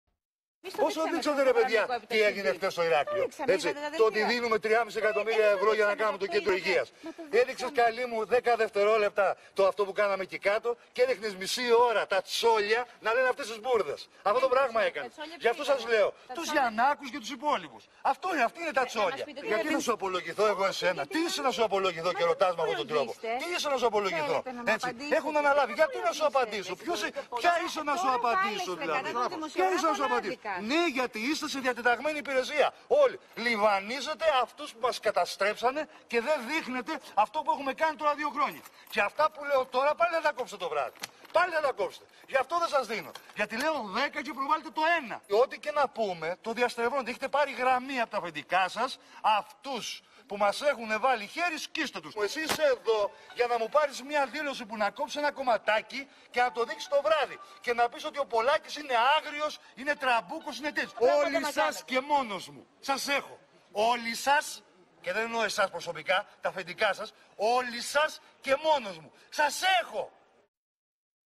Επίθεση Παύλου Πολάκη σε δημοσιογράφους
Νέα επίθεση σε δημοσιογράφους και μέσα ενημέρωσης εξαπέλυσε σήμερα ο αναπληρωτής υπουργός Υγείας Παυλος Πολακης κατά τα εγκαίνια της μονάδας βραχείας νοσηλείας και της ανακαίνισης του φαρμακείου του νοσοκομείου Ερυθρός Σταυρός.